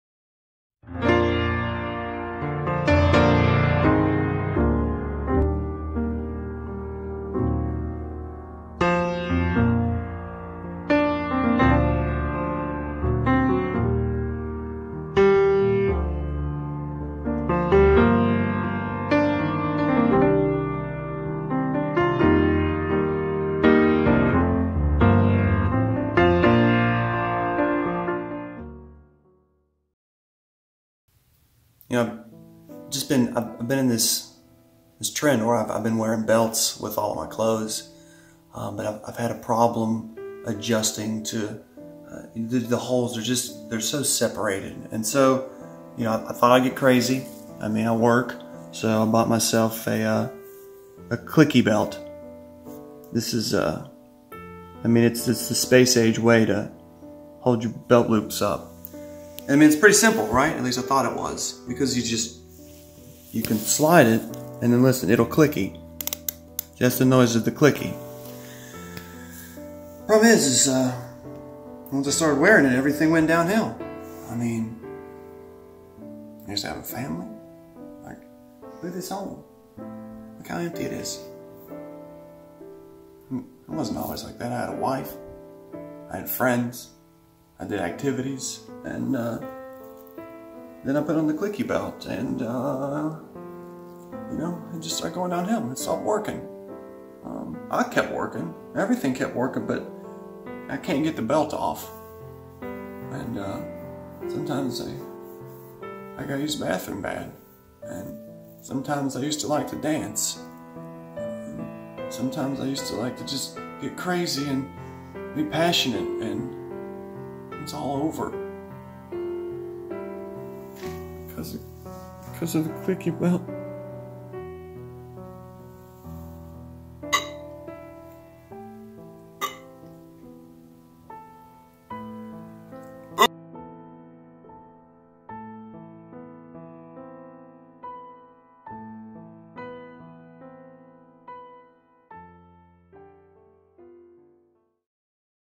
State 35 Sad Product Reviews - ClickyBelt
Sometimes life wants to pants you. Sometimes you just need a belt to help make life tolerable. And, sometimes, said belt can be your false savior. Life is pain. And the Clickybelt is life in this poor gent's case. Share our pain.